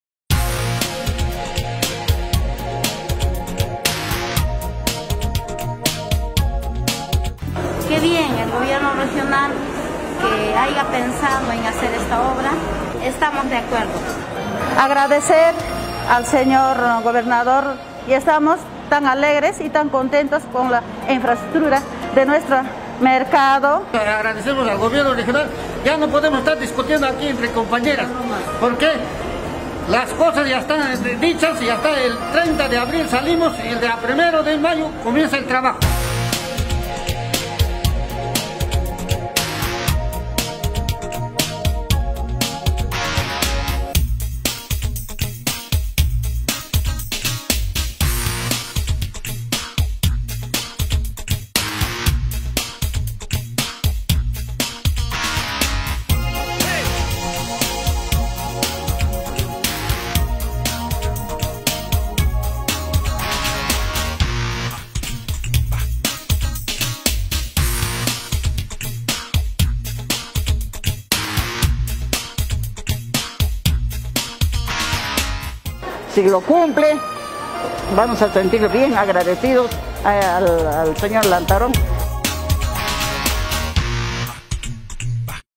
TRABAJADORAS DEL MERCADO CENTRAL OPINAN SOBRE PROYECTO DE REMODELACIÓN DE SU CENTRO DE ABASTOS
OBRA SERÁ EJECUTADA POR EL GOBIERNO REGIONAL
EL GOBERNADOR Y ALCALDE SOCIALIZARON PROYECTO A BENEFICIARIOS
En instalaciones del auditorio Micaela Bastidas de la sede regional, con presencia de decenas de trabajadoras y trabajadores del mercado de abastos y como parte del procedimiento de licencia social, el Gobernador de Apurímac junto al alcalde provincial de Abancay y equipo técnico de la oficina de estudios y proyectos del GORE, socializaron el proyecto de remodelación del mercado central que ejecutará la entidad regional con MÁS DE 5 MILLONES DE SOLES.
EN LA REUNIÓN SE DIO CUENTA DE TODOS LOS COMPONENTES Y SERVICIOS CON LOS QUE CONTARÁ LA MODERNA INFRAESTRUCTURA, CUYOS TRABAJOS SE INICIARÁN EN EL MES DE MAYO Y CONCLUIRÁN EN UN PLAZO DE 4 MESES. EL EQUIPO TÉCNICO DE ESTUDIOS Y PROYECTOS DEL GOBIERNO REGIONAL, DE ACUERDO AL EXPEDIENTE TÉCNICO DEL IOARR, ABSOLVIÓ TODAS LAS DUDAS DE LOS TRABAJADORES, QUIENES MOSTRARON SU SATISFACCIÓN Y RECONOCIMIENTO POR LA CONCRETIZACIÓN DE LA OBRA QUE BENEFICIARÁ A TODA LA POBLACIÓN DE LA PROVINCIA DE ABANCAY.